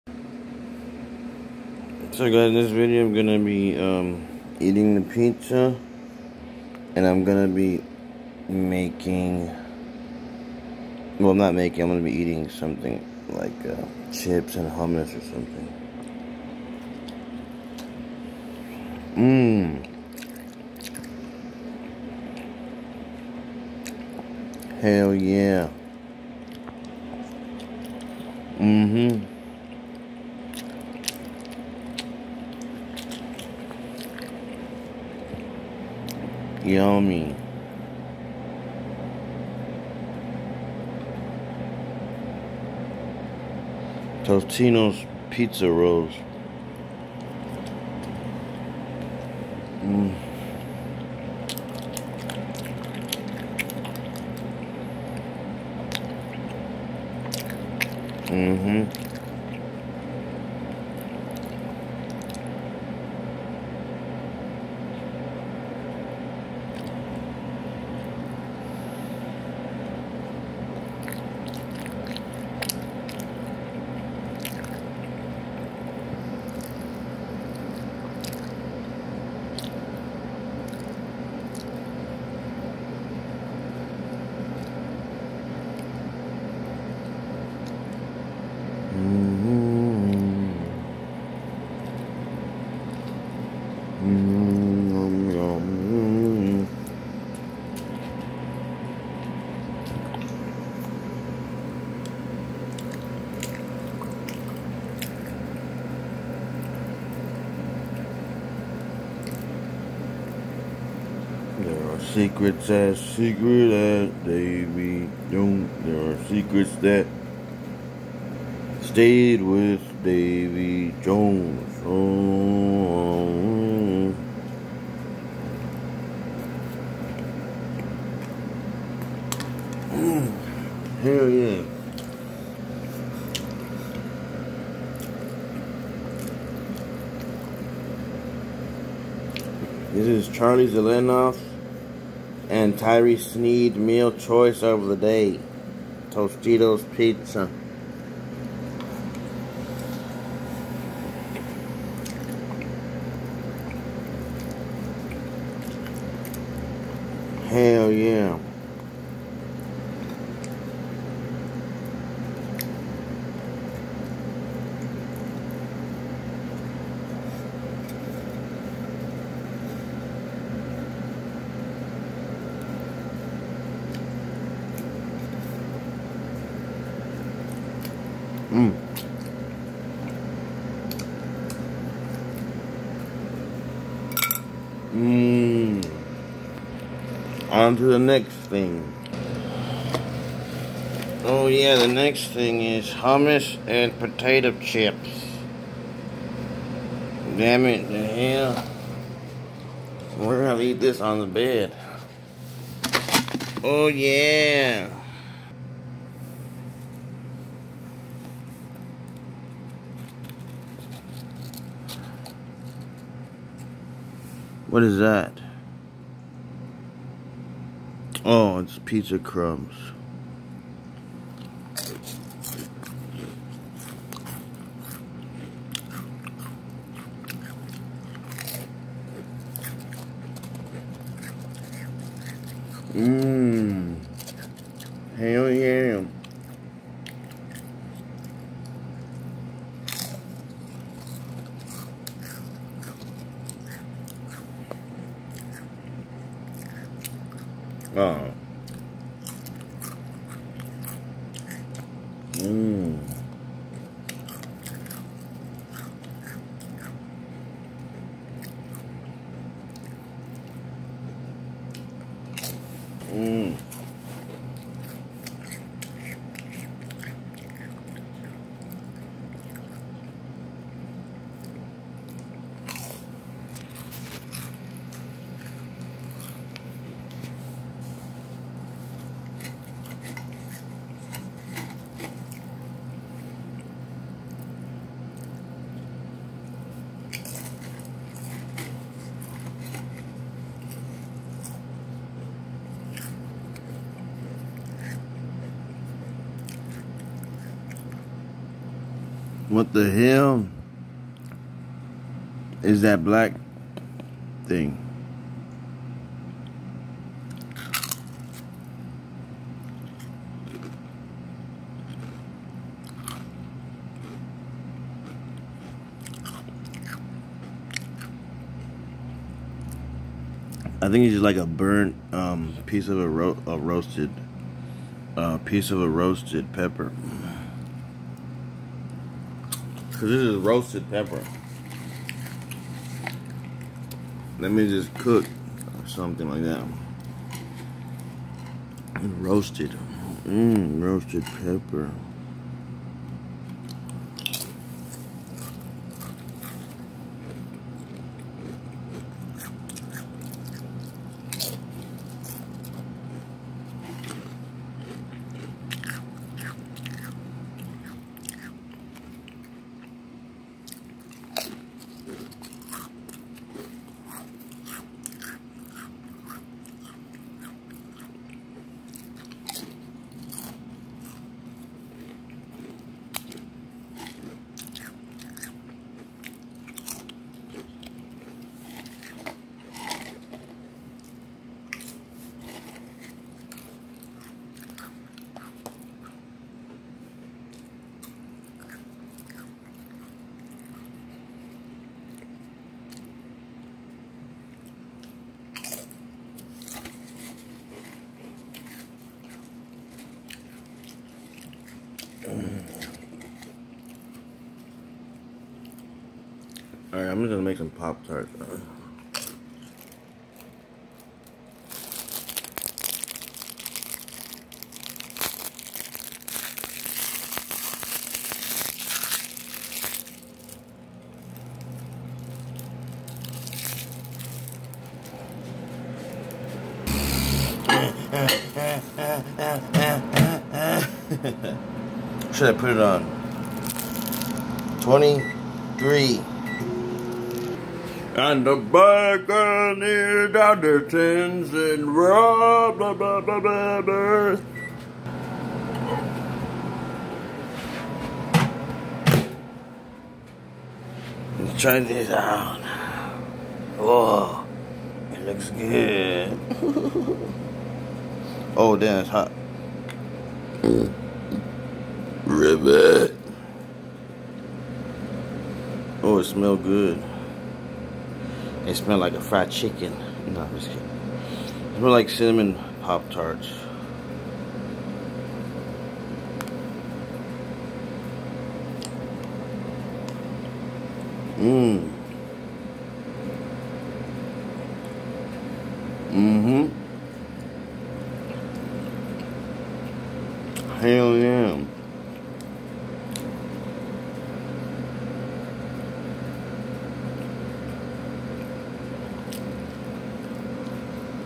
Charlie zelenoff and tyree sneeds meal choice of the day... totinos pizza!!
Jarred 333 follow on everything right now